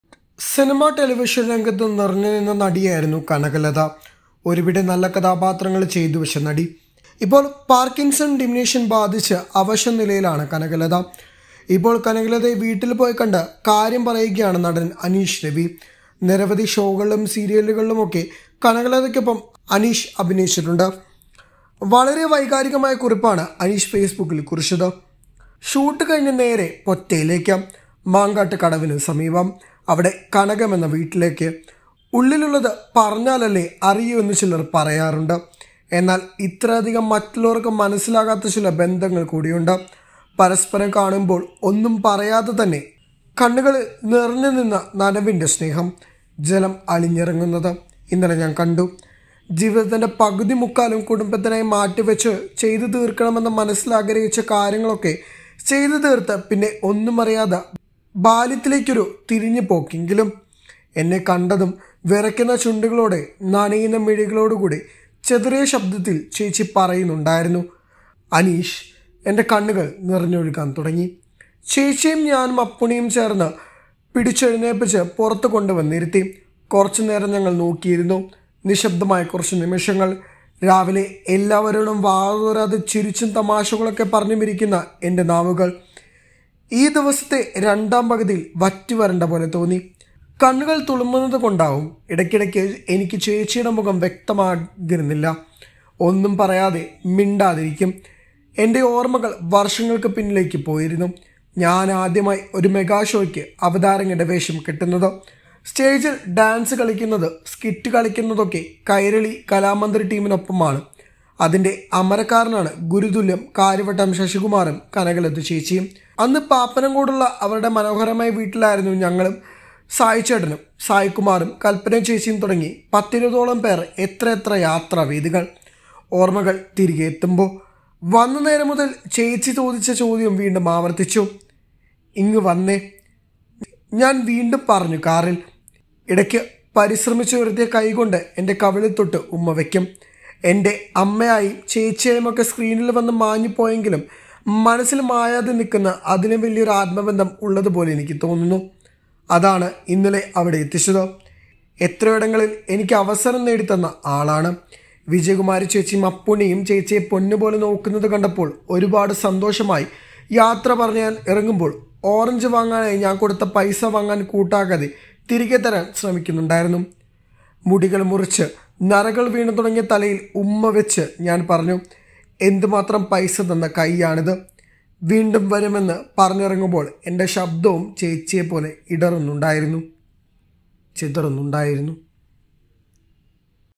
എനിക്ക് ഒരുപാട് പൈസ തന്ന കൈ ആണ് Aneesh Ravi About Kanakalatha Actress
നടി  കനകലതയെ കാണാൻ പോയതിനെപ്പറ്റി വികാരഭരിത പോസ്റ്റുമായി നടൻ അനീഷ് രവി
                ~ED.23~HT.23~PR.16~